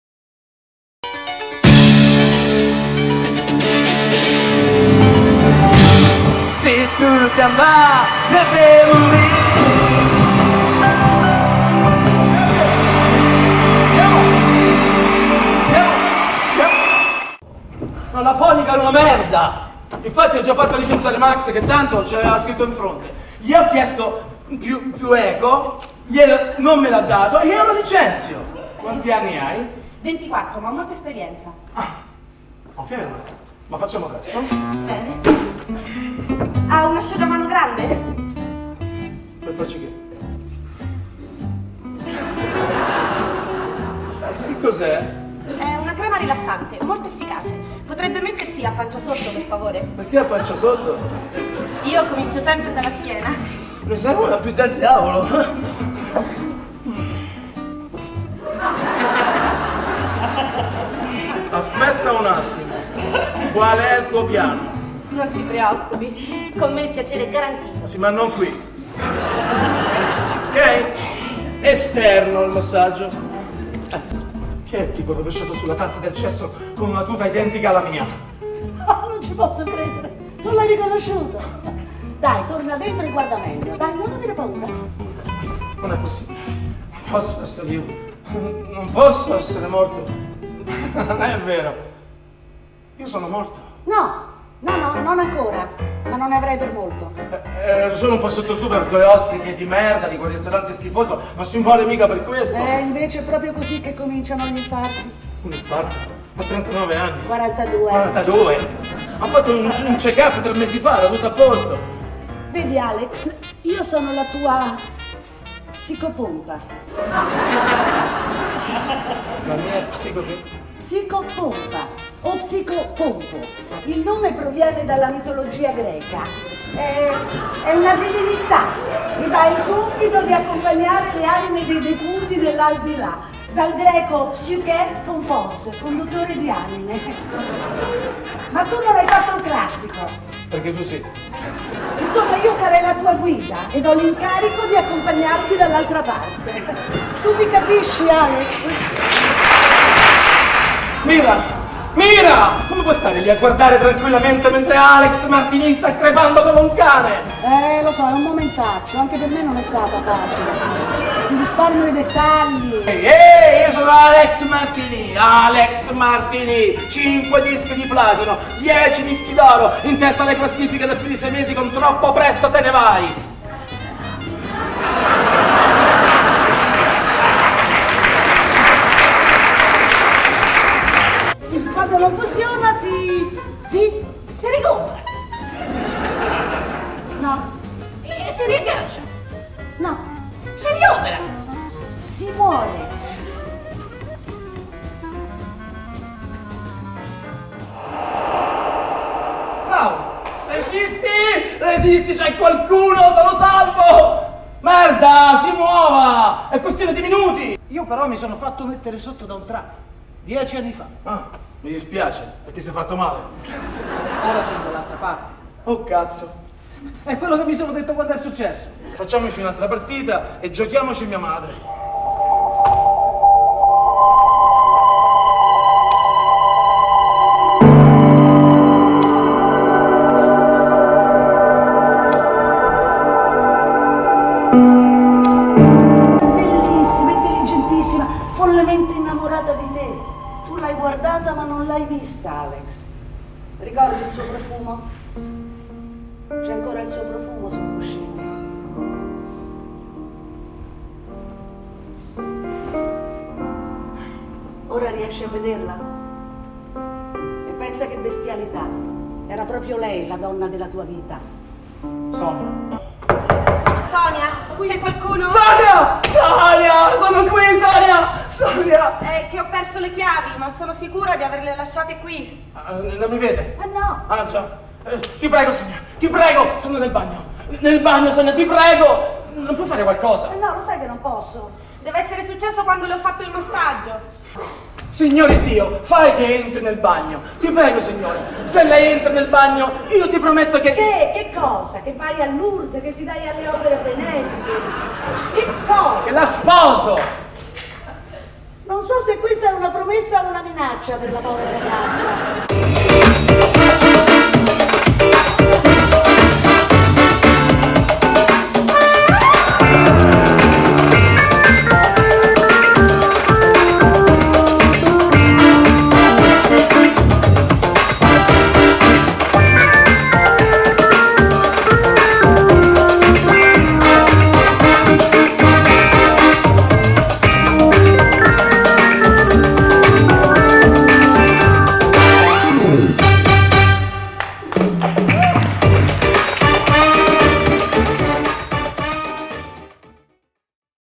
Ultima chiamata - con Pino Quartullo e Paolatiziana Cruciani
Alex Martini (Pino Quartullo), è un cantante rock melodico allapice del successo. Erotico-romantico sul
palco, arrogante e aggressivo nel privato, è convinto che a lui tutto sia permesso. Una sera, rientrando in
albergo rivede il suo corpo riverso nel bagno e gli appare dal nulla una variopinta signora Chantal
(PaolaTiziana Cruciani) che sostiene di doverlo traghettare nellaldilà. Mentre personaggi del passato e
della vita, fantasmi, donne entrano ed escono dalla sua camera, lui rimane bloccato in attesa di sapere se
riuscirà a salvarsi. Essere ricchi e famosi conta ben poco e tutto quello che sembrava urgente ed
importante lascerà posto a sentimenti e valori fino ad allora trascurati. La movimentata e caotica notte
cambierà radicalmente la sua vita con una splendida nuova storia damore.
Unesilarante commedia di Josiane Balasko, grande successo sui palcoscenici di Francia e per le due
stagioni passate anche in Italia con la compagnia di Pino Quartullo e PaolaTiziana Cruciani.